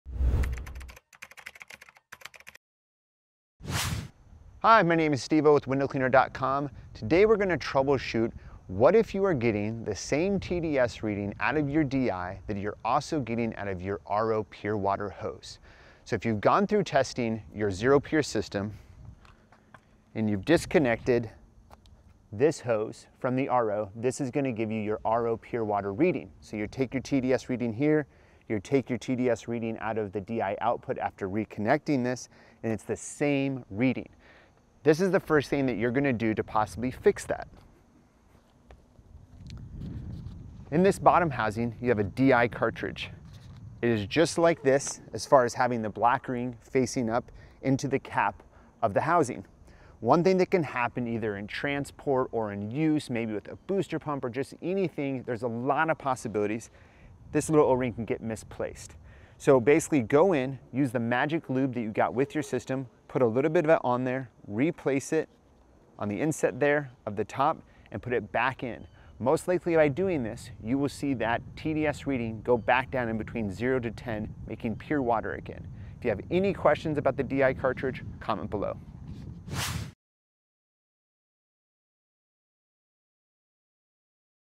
Correct Your TDS Reading with This Simple Method
Troubleshoot a common TDS reading issue at home, or on the job with this quick tutorial from SteveO!

0:00 - Intro
0:07 - The Issue
0:34 - Solution
1:15 - Outro